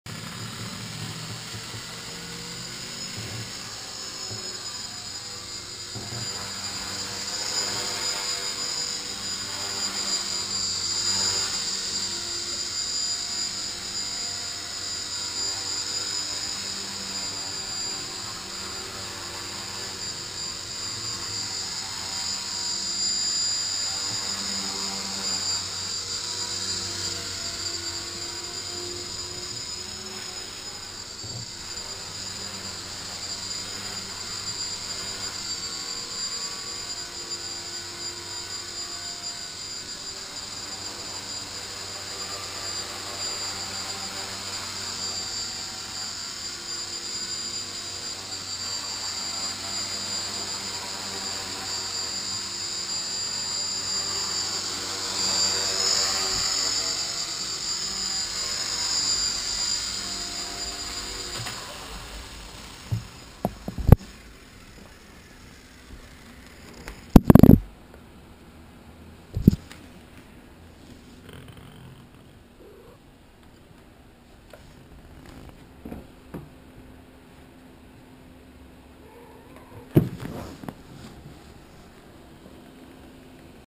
HeliCat
Indoor Cat watching indoor flight
Still waiting on decent flying weather outside

Flying Walkera V120D02s Helicopter
Great little indoor/outdoor Collective Pitch Heli.
Only complaint, the torque-tube cone gear on the tail tends to strip out, not the gear itself but the inside hole of the gear.  Its also an awkward part to replace.  Good news, the part is inexpensive.